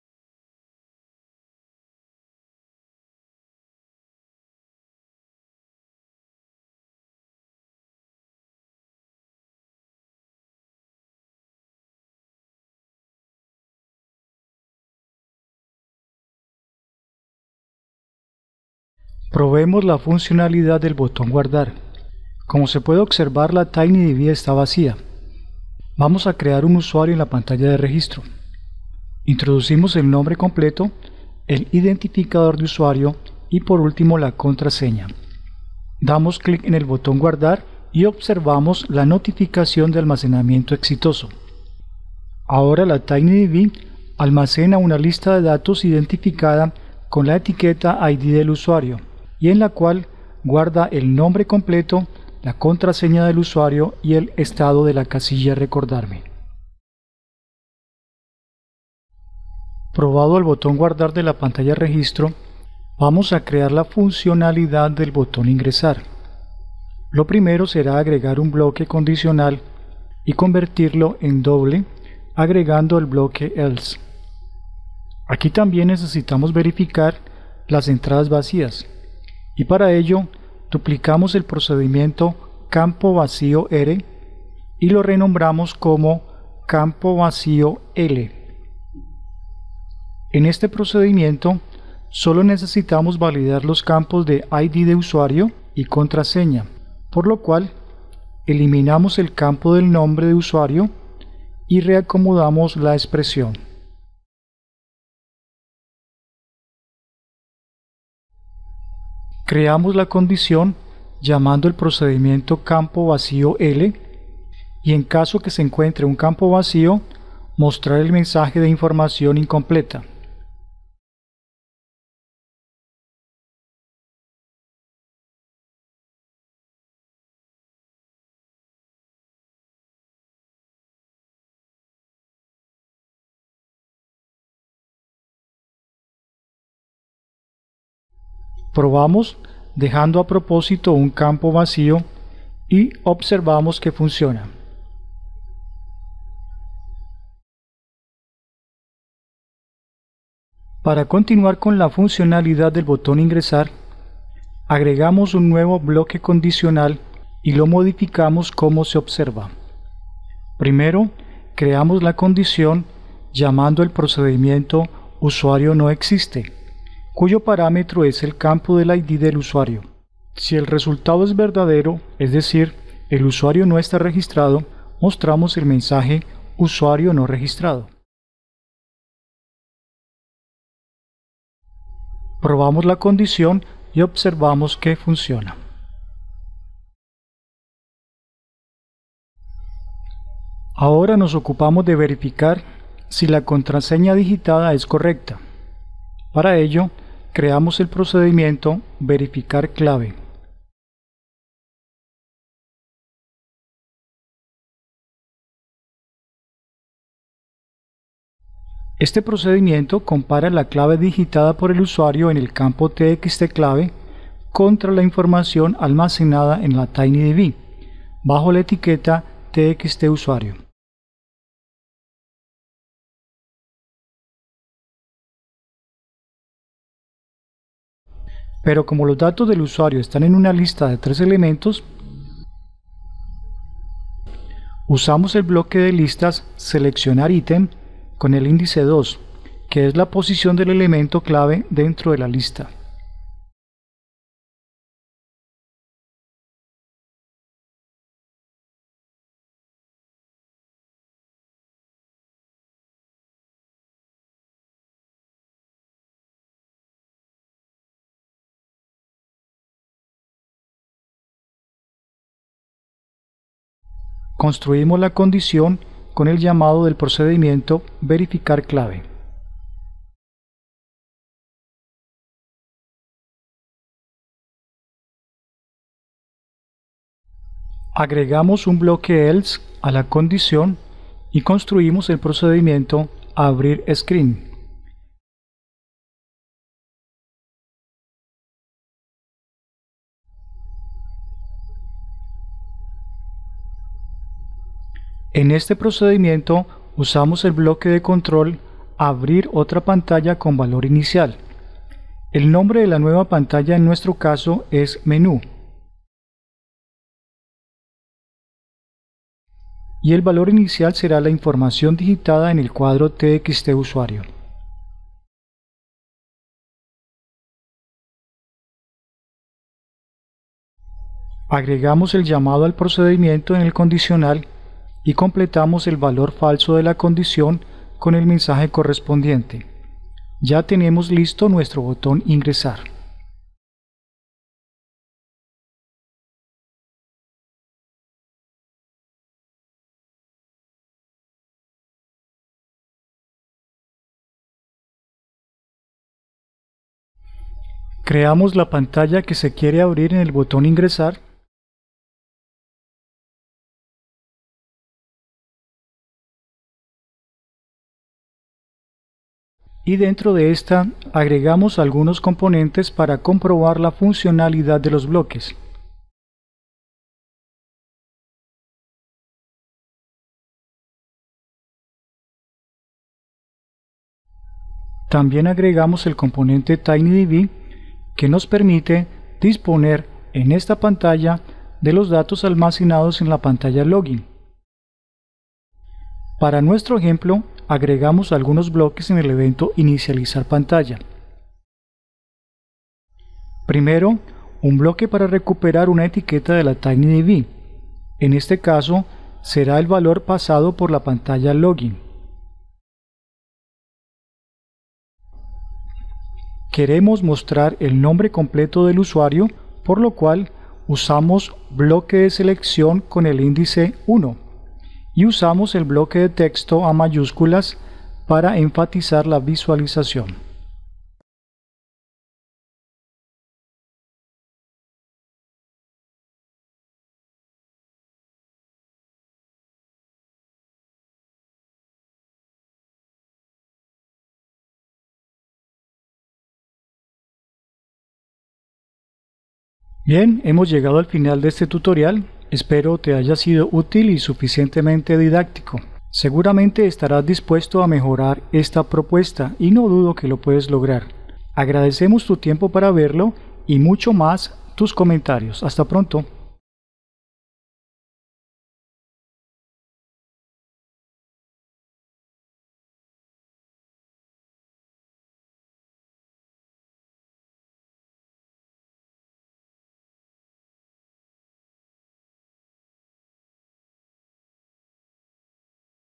App Inventor 2 | Crear Splash, Login y Sign Up - Tutorial #4
Este tutorial te muestra como crear las pantallas Splash, Login y Sign Up de una aplicación usando TinyDB de App Inventor 2. La demostración tiene validación de datos y uso de listas y procedimientos.